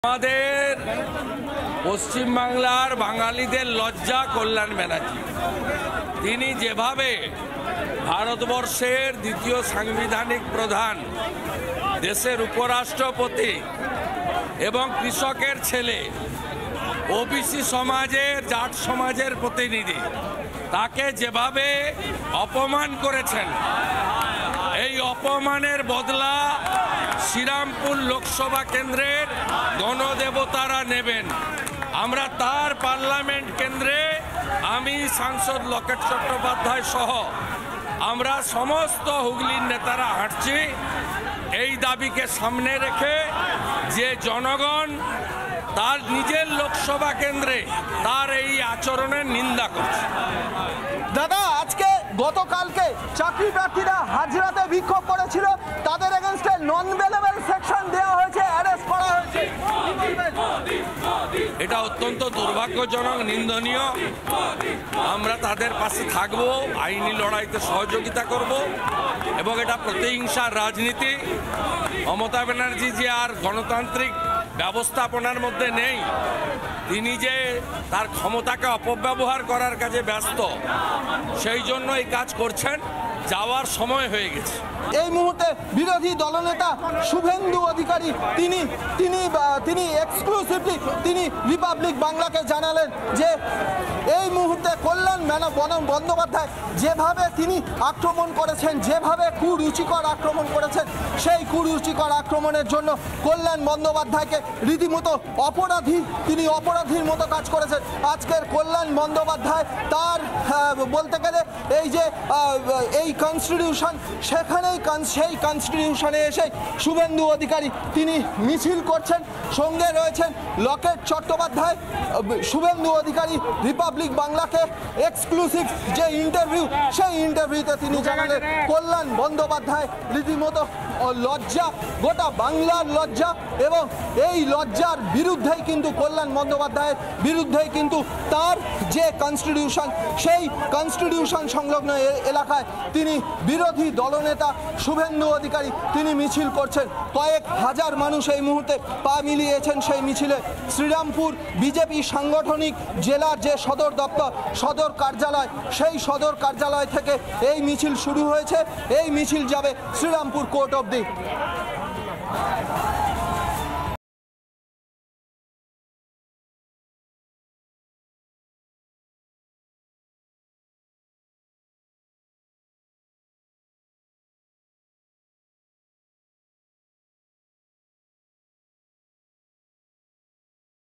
উপরাষ্ট্রপতিকে অপমানের বদলা শ্রীরামপুর লোকসভা কেন্দ্রের গণদেবতারা নেবেন : শুভেন্দু অধিকারীর
দেশের উপরাষ্ট্রপতিকে অপমান, প্রতিবাদে শ্রীরামপুরে মিছিল বিজেপির। মিছিলের নেতৃত্বে শুভেন্দু অধিকারী। 'অপমানের বদলা শ্রীরামপুর লোকসভা কেন্দ্রের গণদেবতারা নেবেন', রিপাবলিকে প্রতিক্রিয়া শুভেন্দু অধিকারীর

রিপাবলিক স্বাধীন।
রিপাবলিক নির্ভীক।
আপনিই রিপাবলিক।